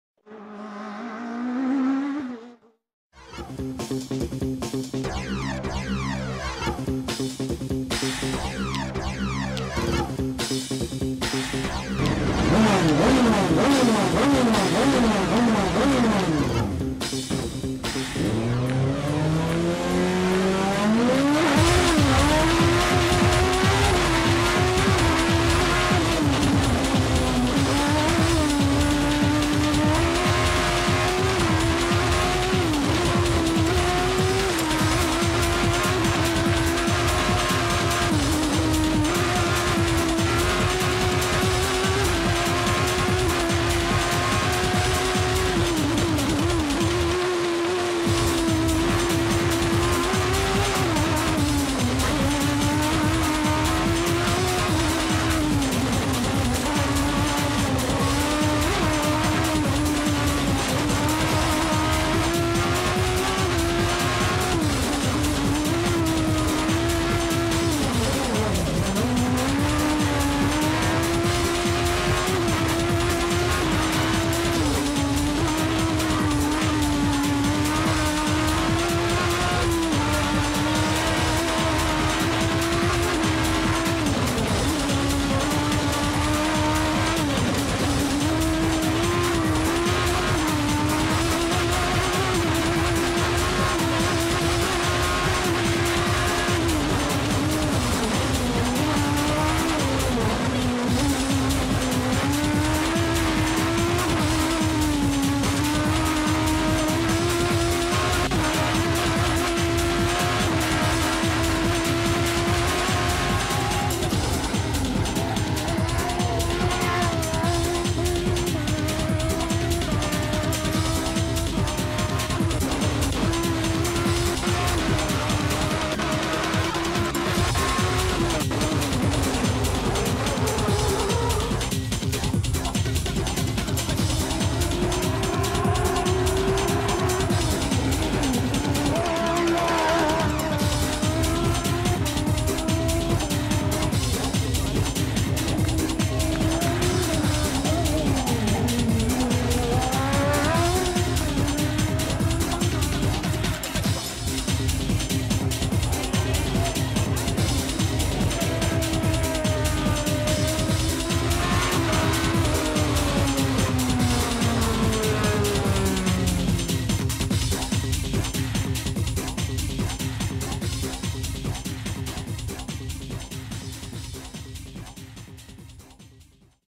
OTS VIDEO SPORT: Si es que van como Locos 3 - onboard 3
Montaje de cámaras interiores en la Subida al Mármol (Almería) 2006:
Alberto García Mateos (Speed Car GT 1000).
Incluido en el recopilatorio "Si es que van como Locos 3".